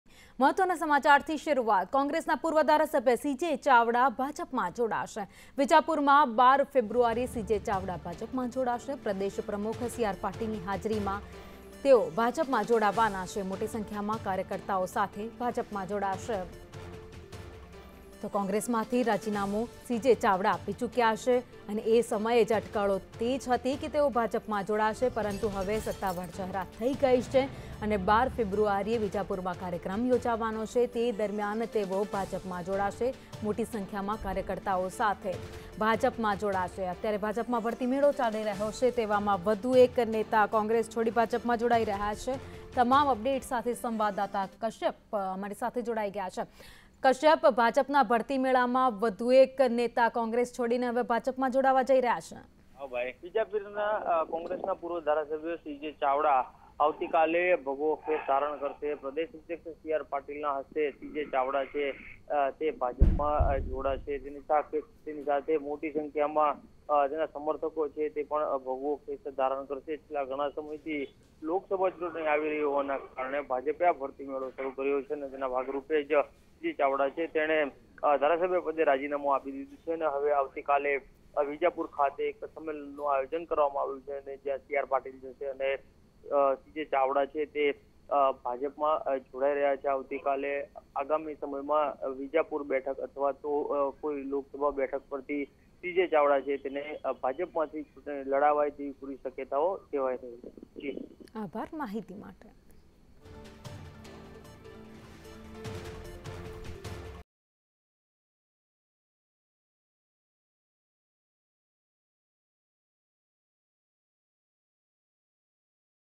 Breaking News | કોંગ્રેસના પૂર્વ ધારાસભ્ય સી જે ચાવડા જોડાશે ભાજપમાં | C.J. Chavda | BJP
કોંગ્રેસના પૂર્વ ધારાસભ્ય સી.જે.ચાવડા જોડાશે ભાજપમાં
વિજાપુરમાં 12 ફેબ્રુઆરીએ સી.જે.ચાવડા ભાજપમાં જોડાશે
પ્રદેશ પ્રમુખ સી.આર.પાટીલની હાજરીમાં વેલકમ પાર્ટી
મોટી સંખ્યામાં કાર્યકર્તાઓ સાથે જોડાશે ભાજપમાં